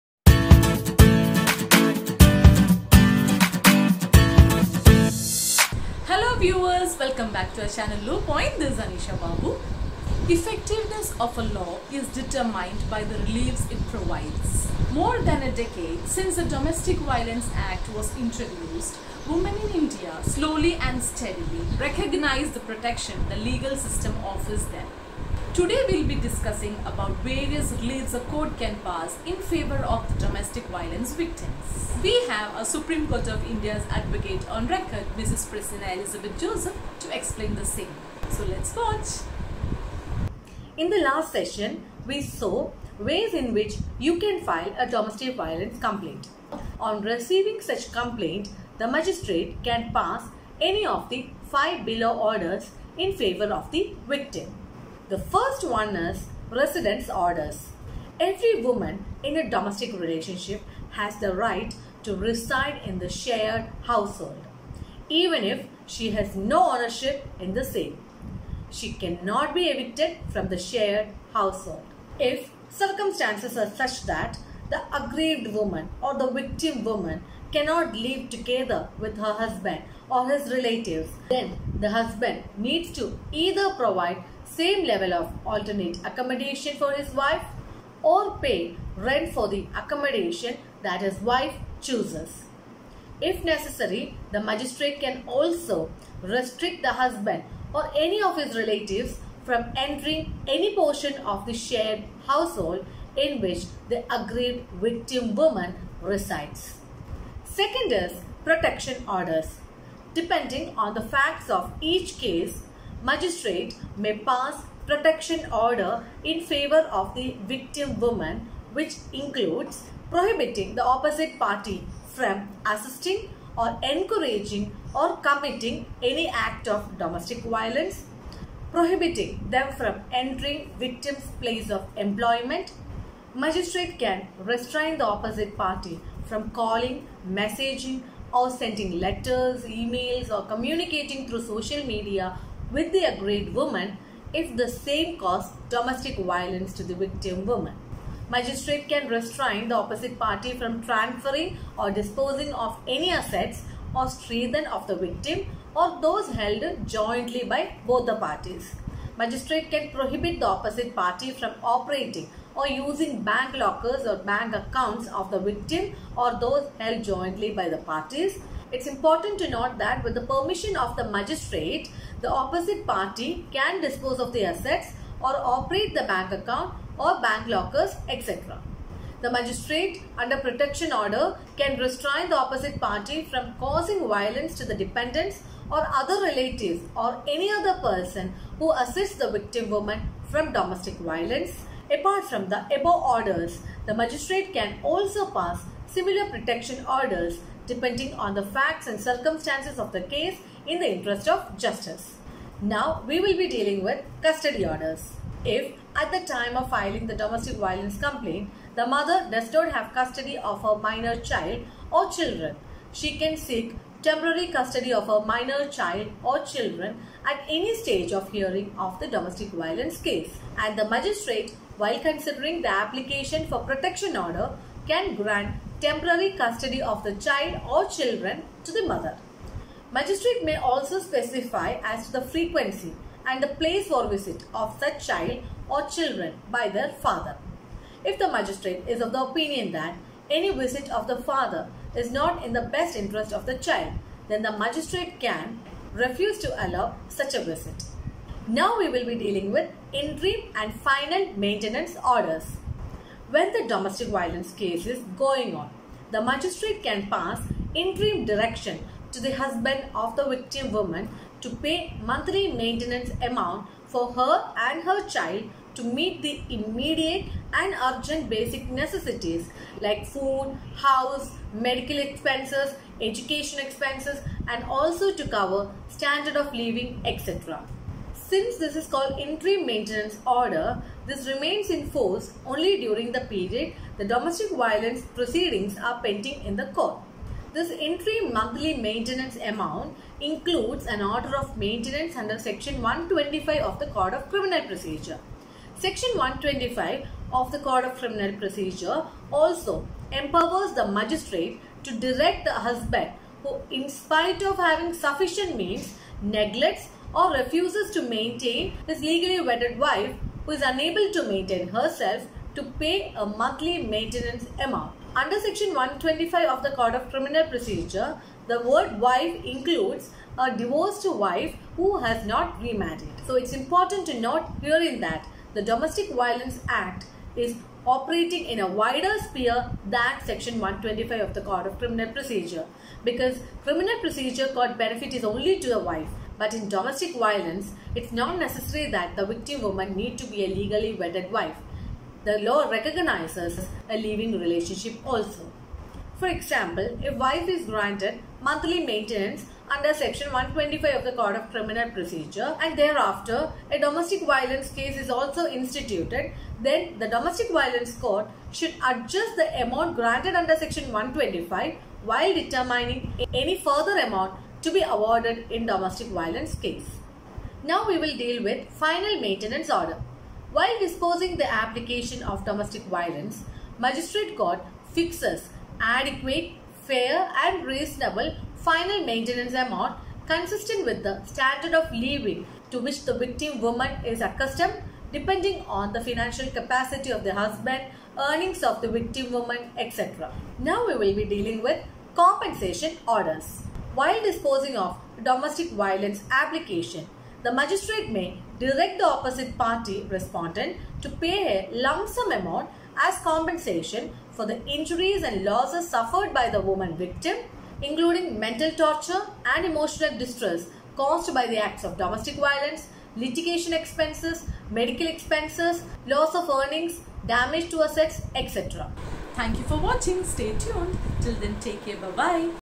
Domestic violence Orders
Effectiveness of a Law is  determined by the reliefs it provides.
More than a decade since the Domestic Violence act was introduced, women in India has slowly and steadily recognise the protection, the legal system offers them.
Today we will be discussing about the various reliefs that the Court can pass in favour of victims of Domestic violence cases. We have our Supreme Court of India's advocate on record Mrs. Praseena Elizabeth Joseph to explain the same.